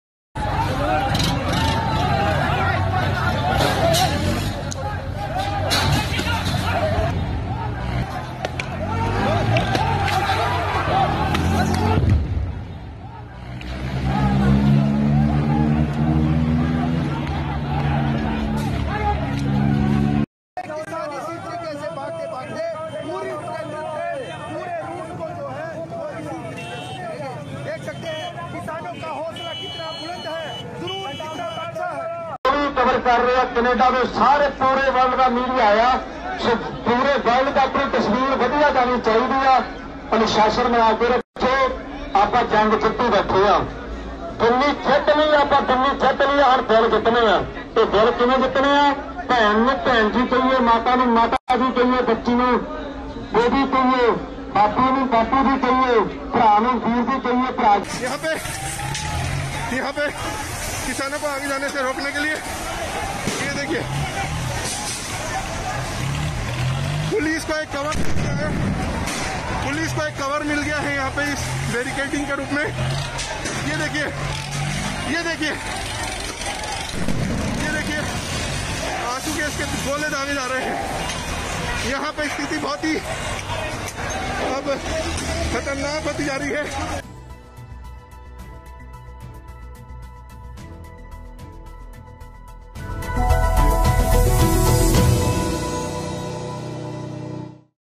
Clashes as farmers hold ‘tractor rally’ on India’s Republic Day | VNS Live
Indian farmers protesting against agricultural reforms on India’s 72 republic day on Tuesday amid clashes with police in the capital, who fired tear gas to restrain them.
Protesters defied tear gas shelling and continued to march on the highway as they raised slogans against the government.

VNS Live | Video News Service